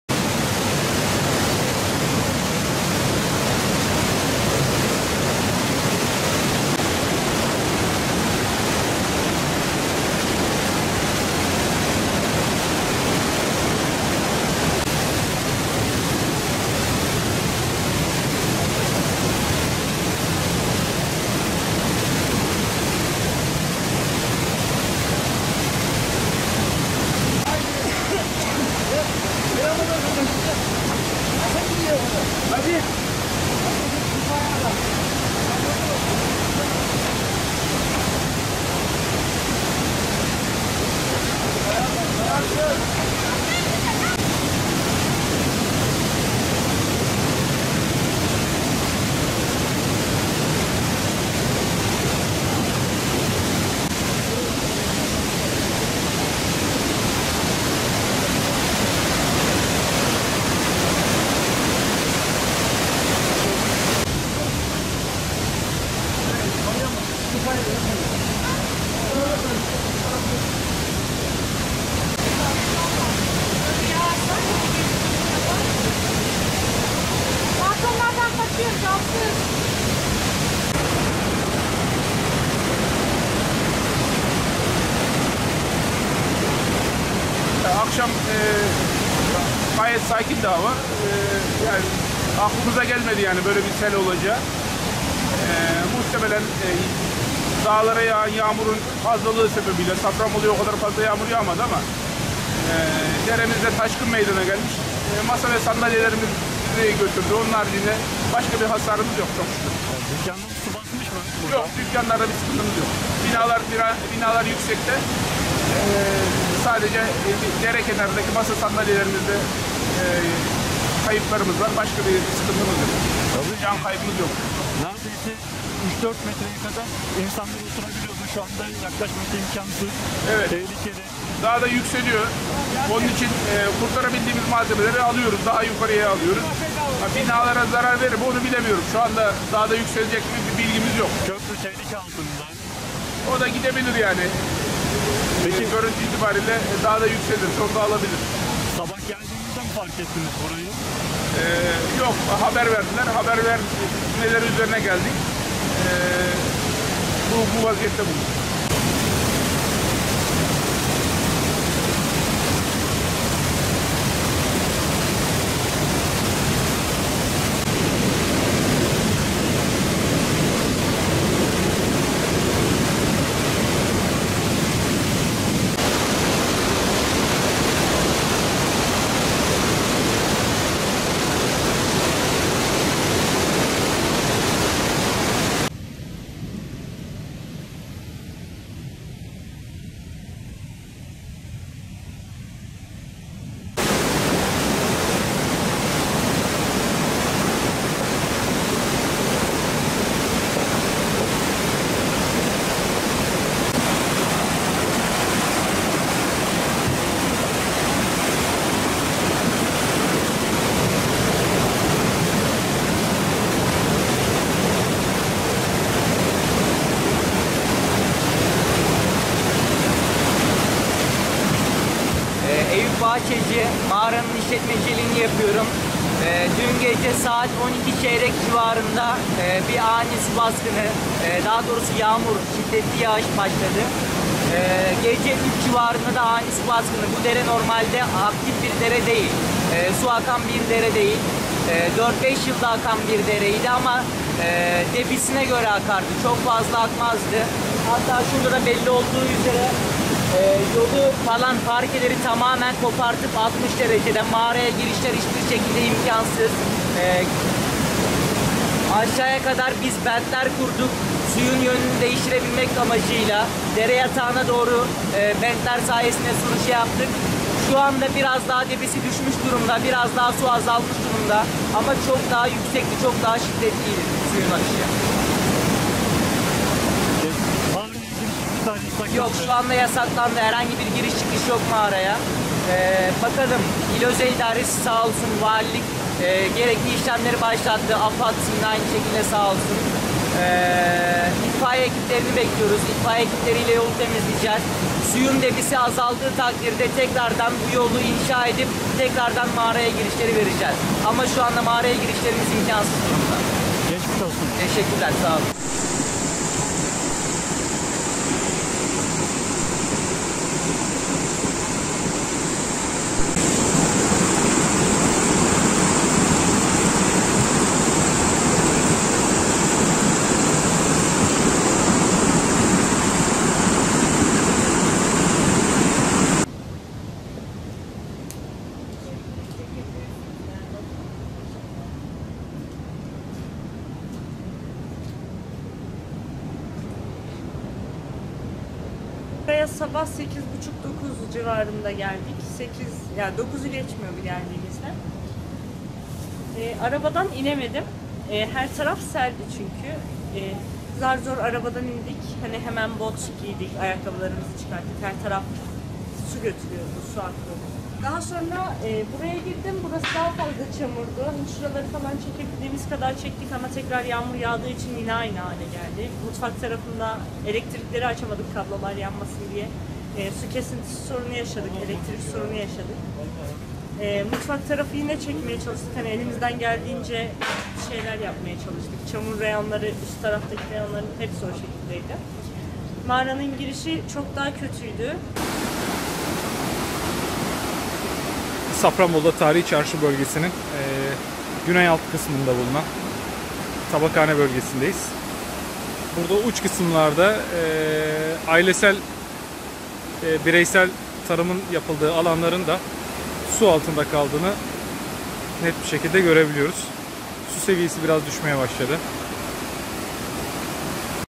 Sağanak sonrası dereler taştı, dükkanlar tehlikeyle karşı karşıya kaldı
Karabük’ün Safranbolu ilçesinde etkili olan sağanak yağış nedeniyle taşan dereler dükkanları tehlikeye atarken, masa ve sandalyeler sulara teslim oldu.